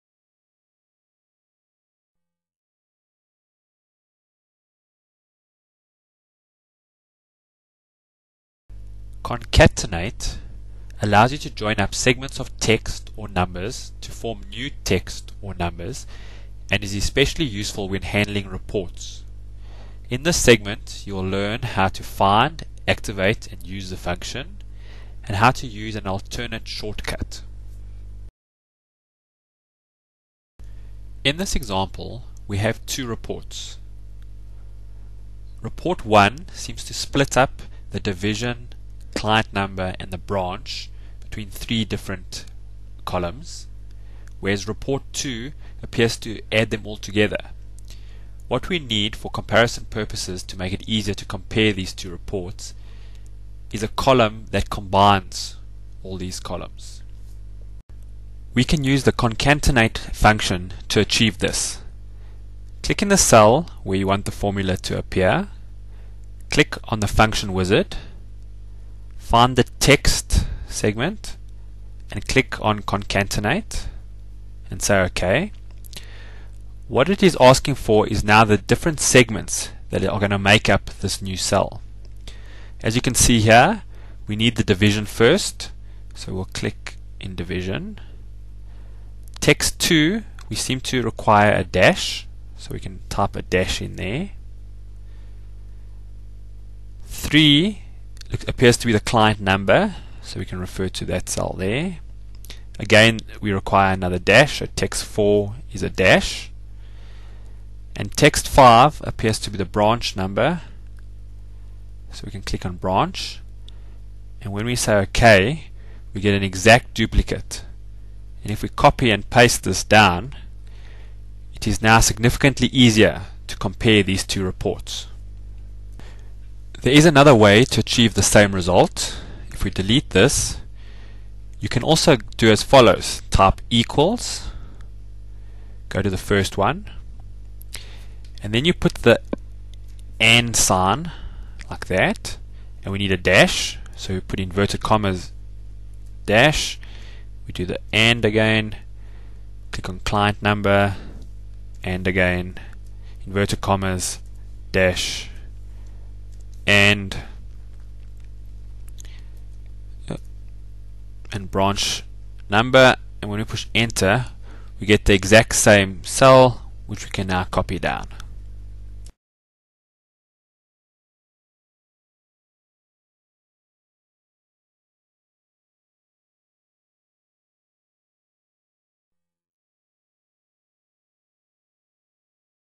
Merge text from multiple cells together in Excel 2003
CONCATENATE allows you to join up segments of text or numbers to form new text or numbers, and is especially useful when handling reports.